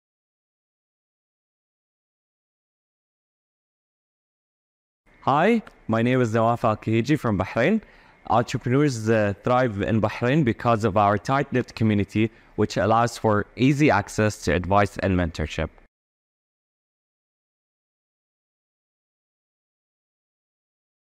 Entrepreneurs Thrive Here Bahrain
Entrepreneurs are living and thriving in Bahrain. Join Global Entrepreneurship Week Nov. 13-19, 2023.